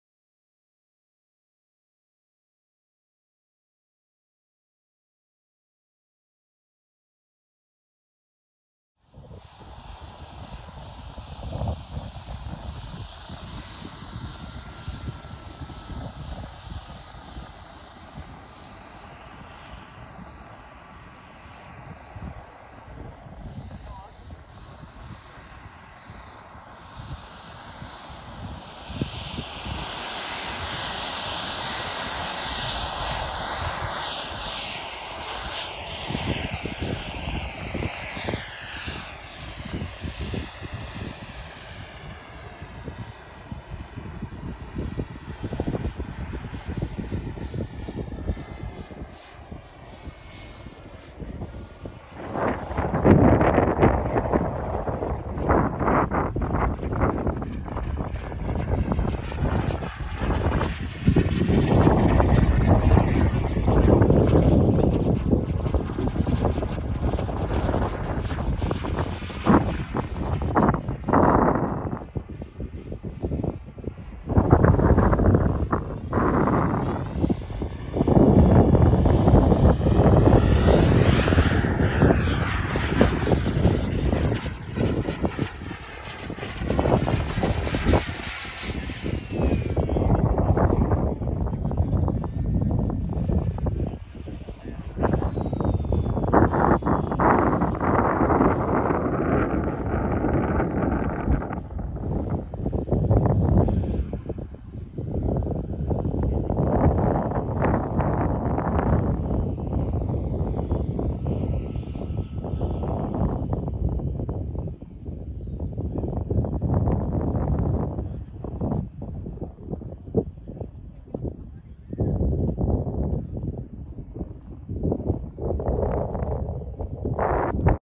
Tomas Leijon from Sweden flies his Opus-Jet CC BG at Karlskoga JetPower 2022.
Tomas Leijon flies his Opus-Jet CC BF with a Xicoy X-45 at Karlskoga JetPower in Sweden 2022. This is the fourth flight with this model. Top speed about 300 Mph+.